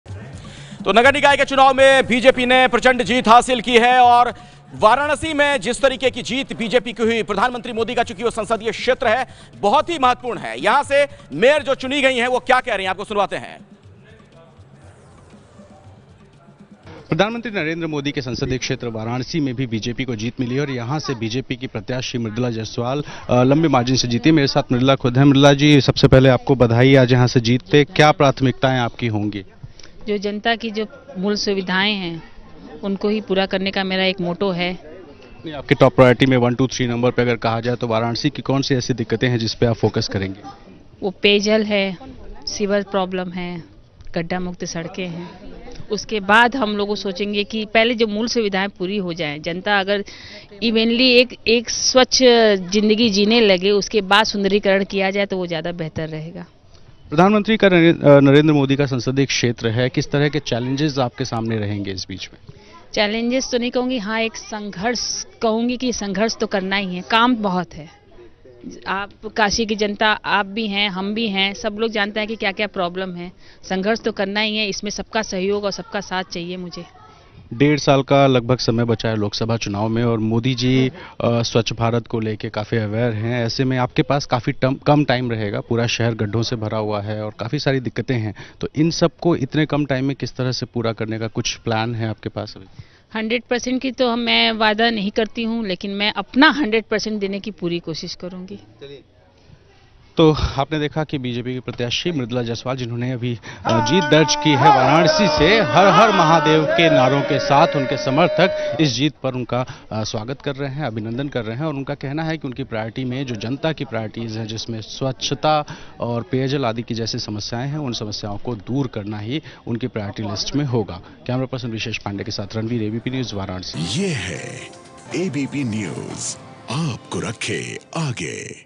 वाराणसी से बीजेपी कैंडीडेट मृदुला जायसवाल ने जीत के बाद बताईं अपनी योजनाएं | ABP News Hindi
वाराणसी से बीजेपी कैंडीडेट मृदुला जायसवाल ने जीत के बाद बताईं अपनी योजनाएं